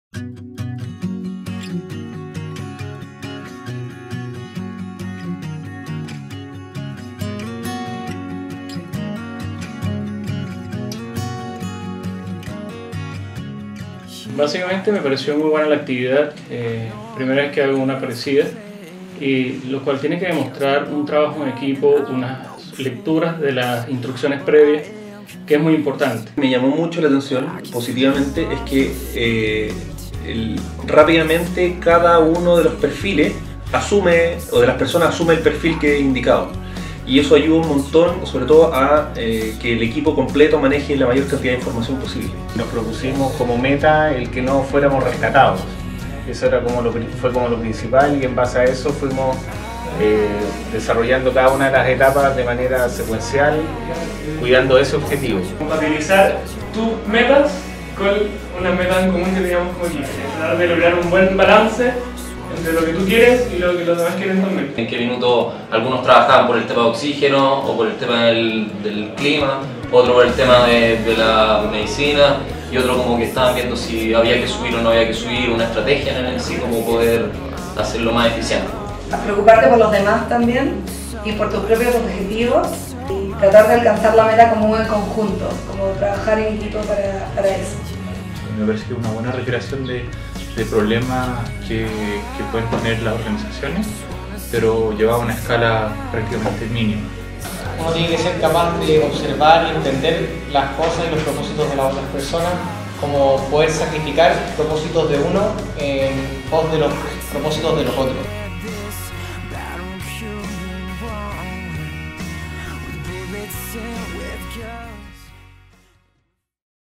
Aseguramiento del Aprendizaje de la Escuela de Negocios 2012, Universidad Adolfo Ibáñez
Testimonios de los participantes de la jornada de Aseguramiento de la Escuela de Negocios de la UAI, "Evaluando las competencias transversales de nuestros estudiantes"